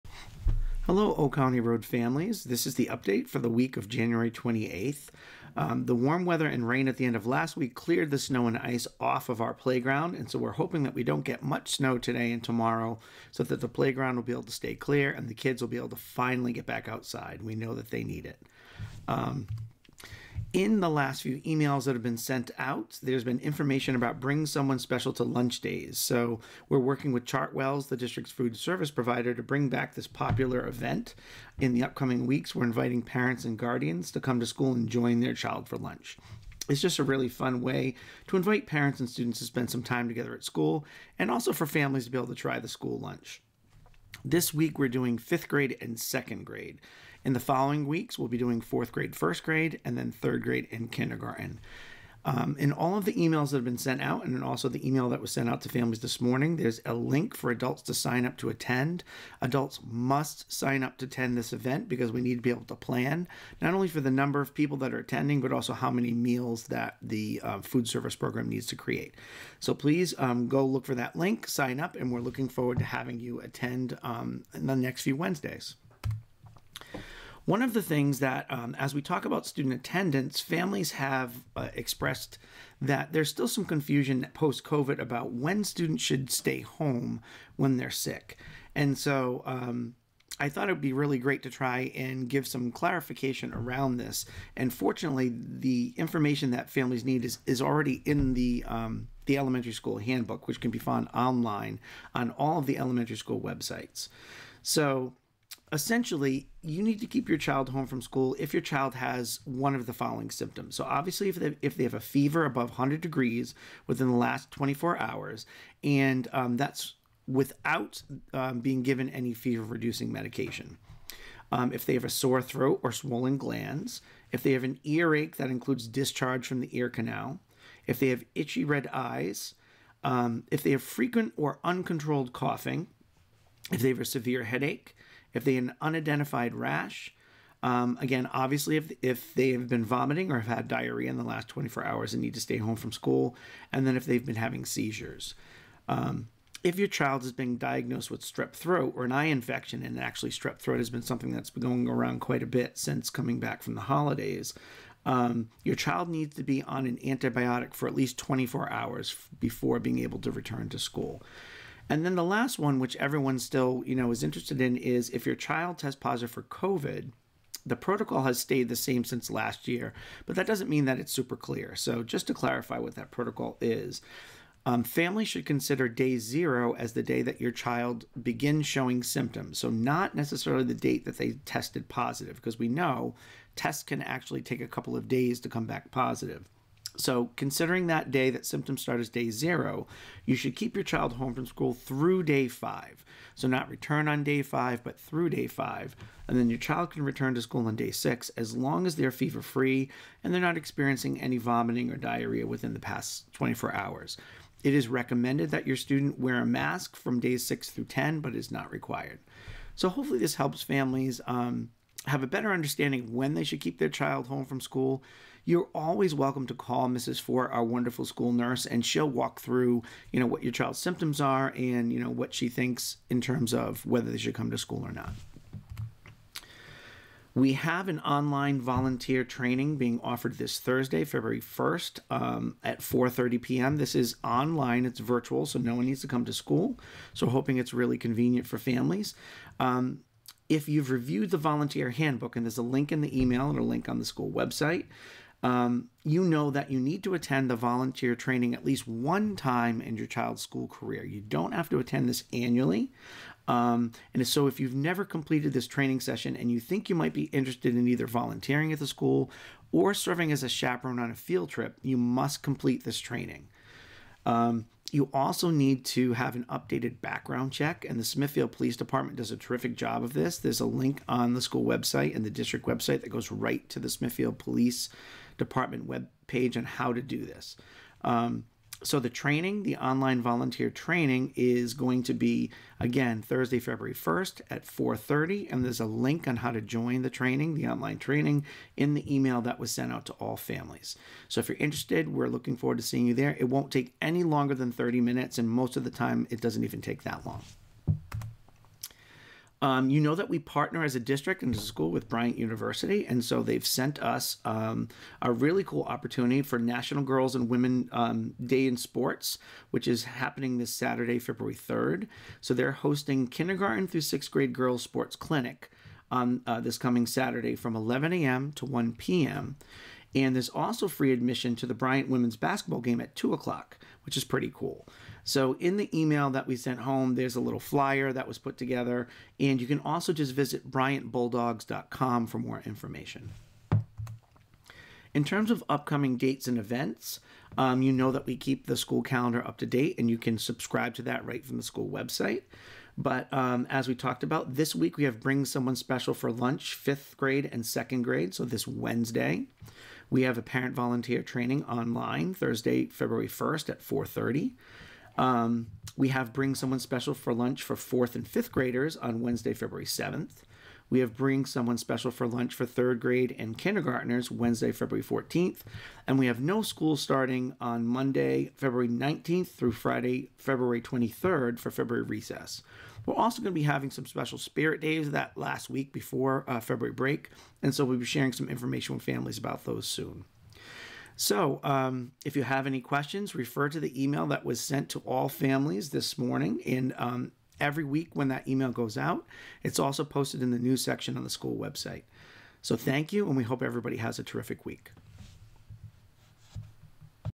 Update for the Week of January 28th
00:00 - Introduction
00:19 - Bring Someone Special to Lunch
01:16 - When Should Students Be Kept Home from School Due to Illness
03:46 - Online Volunteer Training Offered on Thursday, 2/1 at 4:30 PM
04:59 - Bryant University National Girls & Women in Sports Day on Saturday, 2/3
05:34 - Upcoming Events and Dates